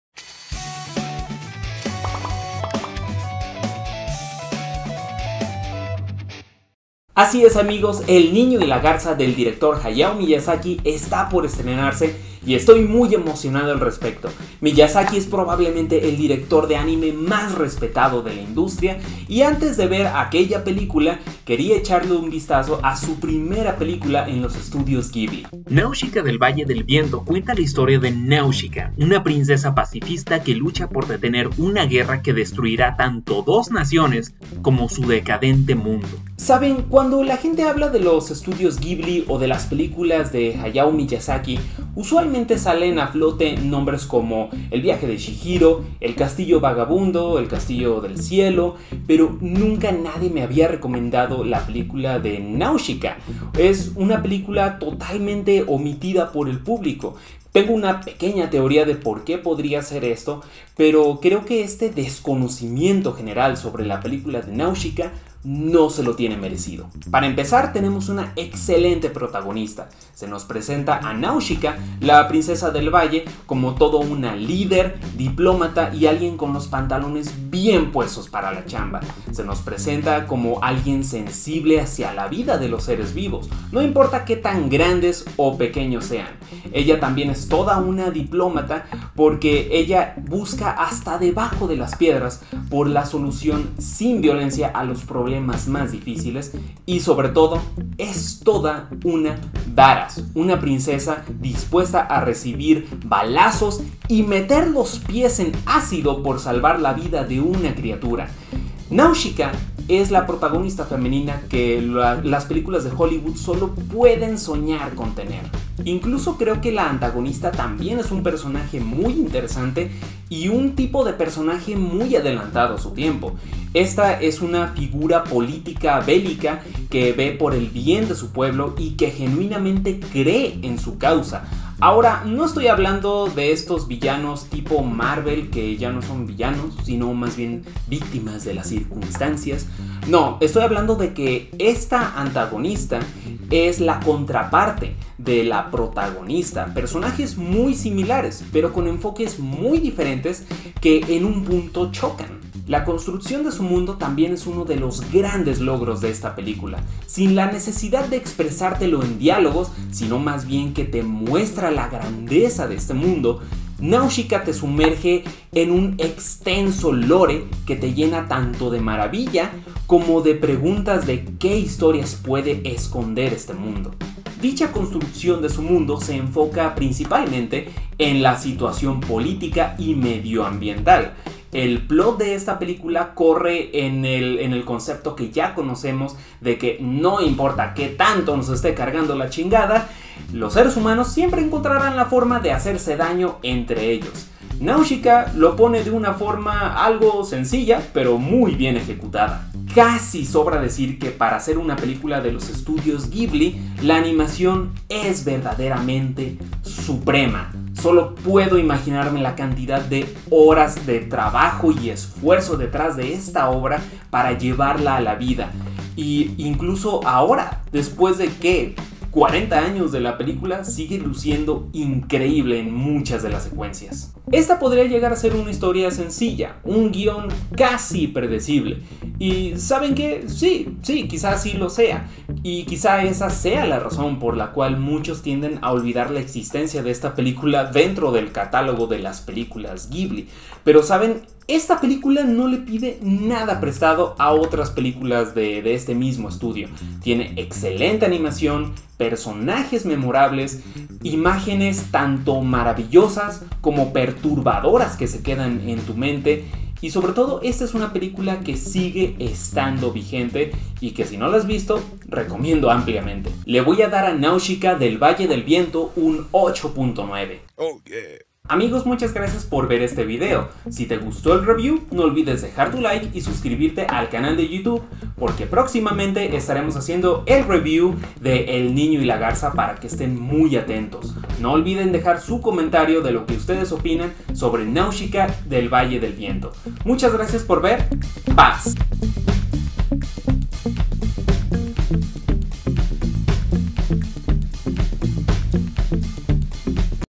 NAUSICAÄ Del Valle Del Viento (1984) Reseña y Opinión
Únete al análisis de Nausicaä del Valle del Viento (Kaze no tani no Naushika), una película de ánime dirigida por Hayao Miyazaki y protagonizada por Sumi Shimamoto, Mahito Tsujimura, Hisako Kyôda. De los Estudios Ghibli !

Con su anfitrión Alan Lee!

CRÍTICA / REACCIÓN / FINAL EXPLICADO / VALE LA PENA? / OPINIÓN / REVIEW / RESUMEN Completo /ANÁLISIS